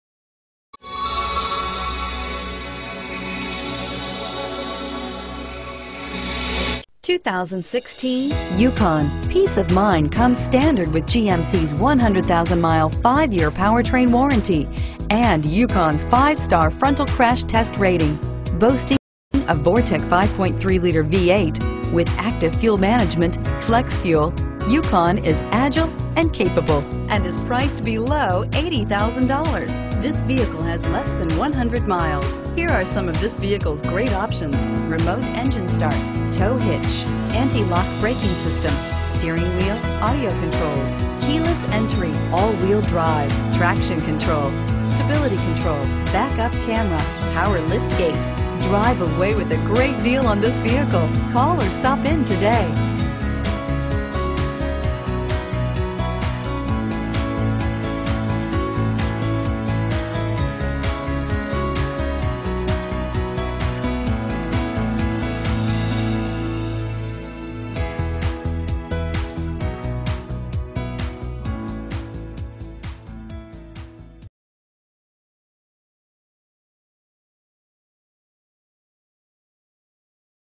2016 GMC Yukon San Diego CA 616467
2016 GMC Yukon Denali

Looking for the right sport utility? Today could be your lucky day. With 7 miles, this White Frost Tricoat, 2016 GMC Yukon equipped with Automatic transmission could be yours. Request more information and set up a test drive right away.